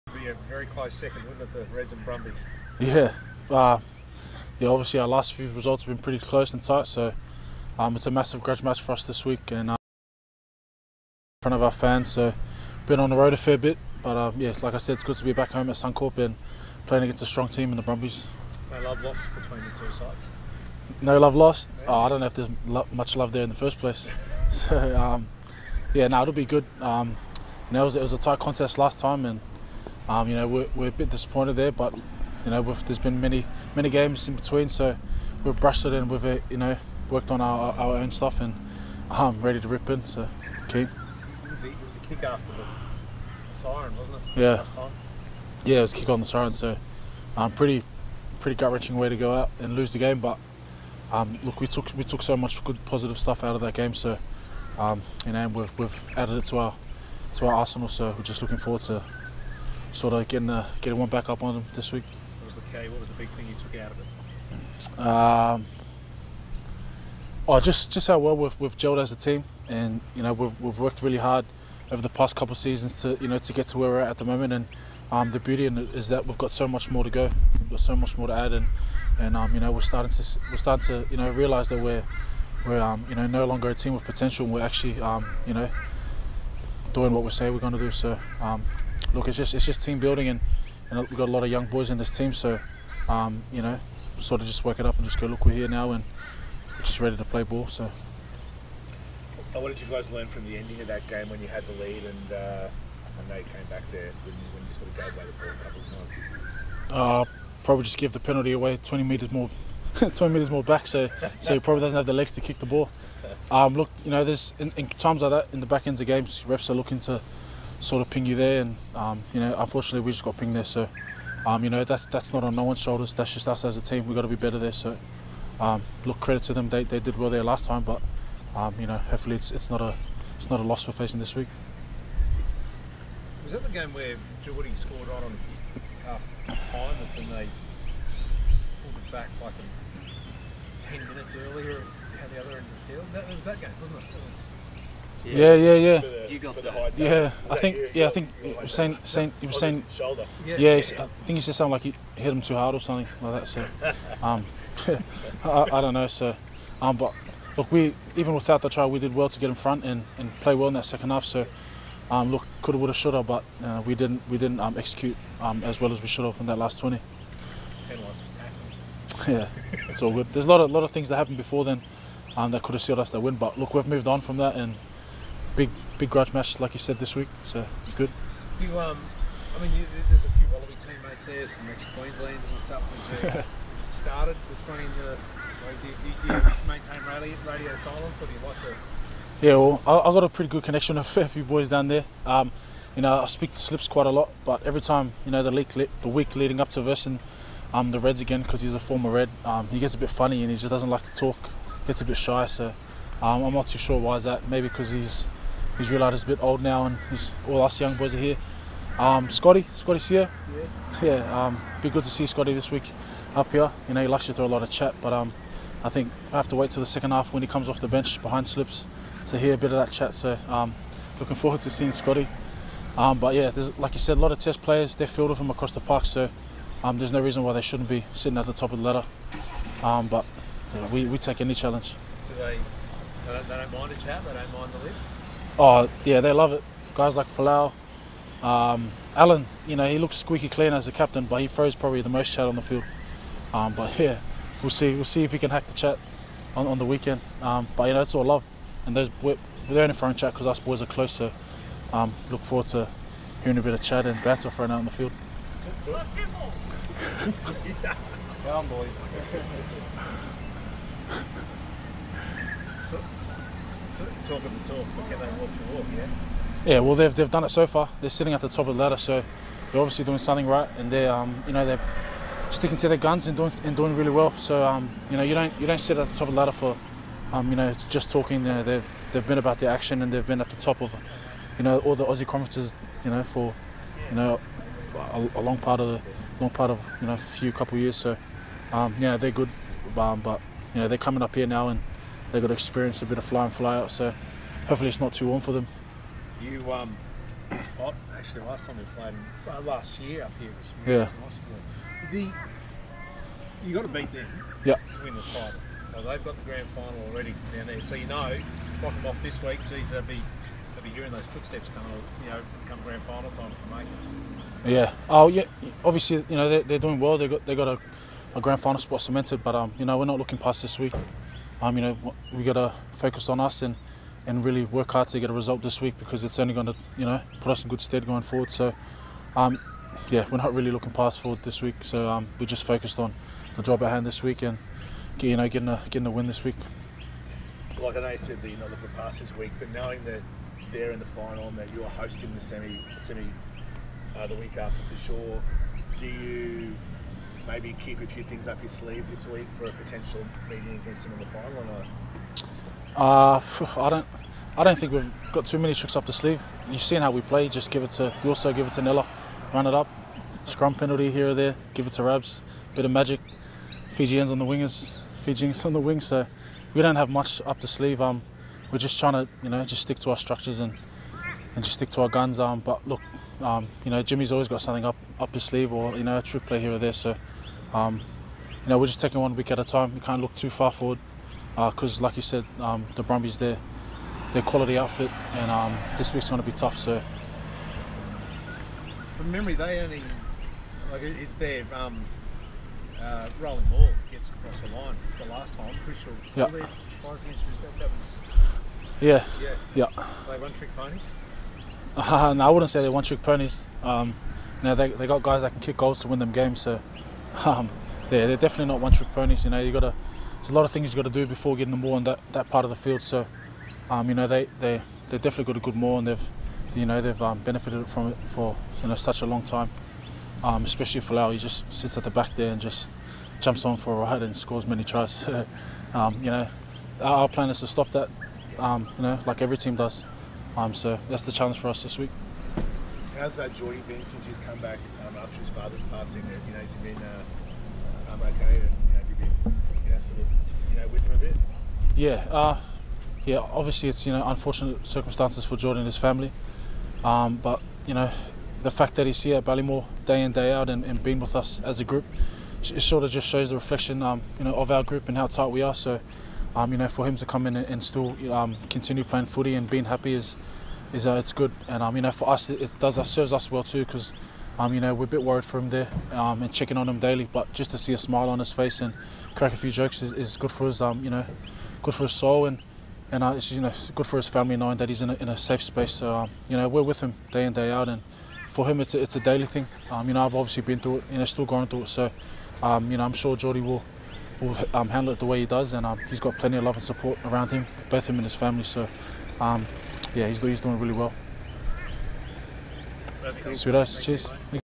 200901 - Lukhan Salakaia-Loto
Lukhan Salakaia-Loto talks to the media. Footage supplied by the Queensland Rugby Union.